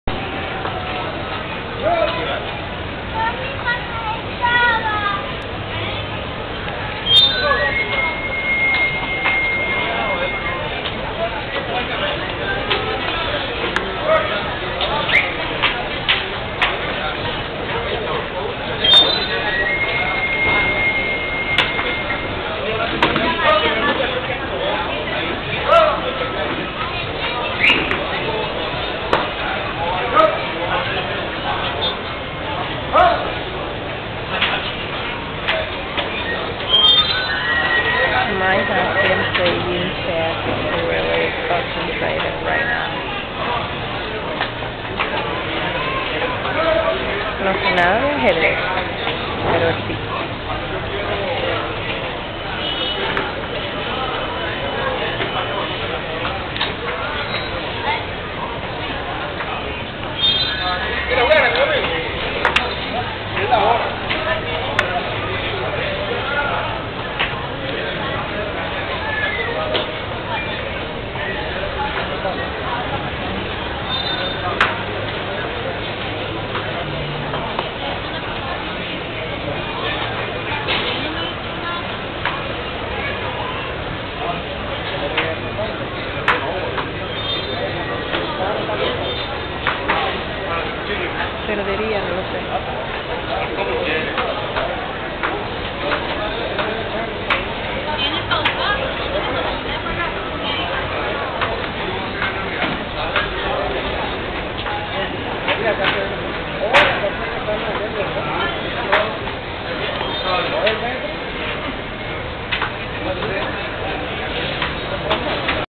ALLAN PLAYING CHESS
Allan vega playing at Sabana Grande Plaza...!